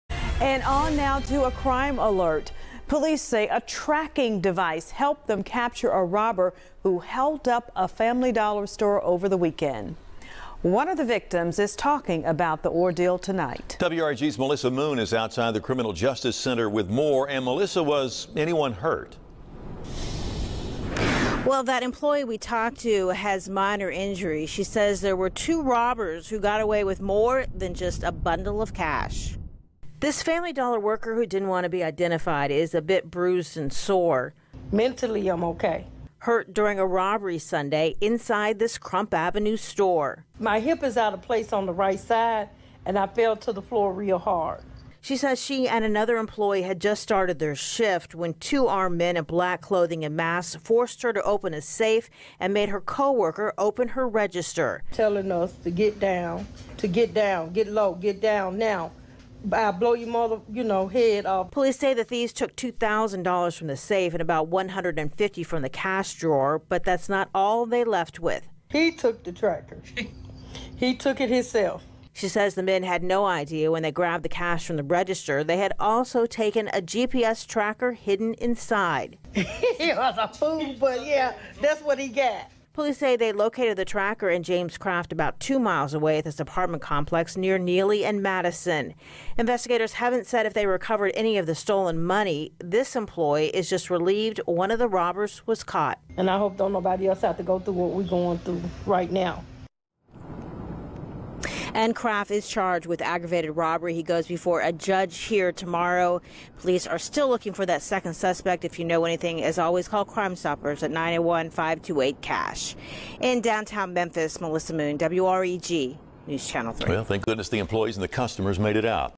Hidden tracker helps police catch Family Dollar robber
Our channel is about crime alert videos for everybody to watch . We cover lots of scary stuff such as crime, murder and crime stories .